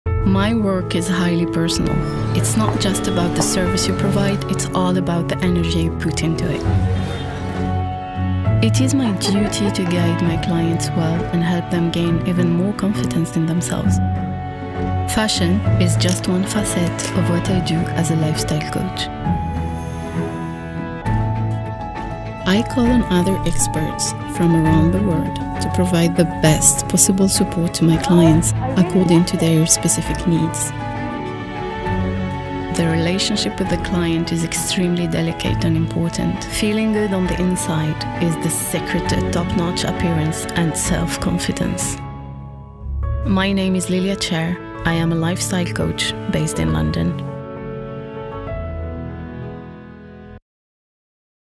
Fashion Promotional Film (Lofty Kaz Vision)
Promo film I made for a small London business owner Lilya Chair as a lifestyle Coach.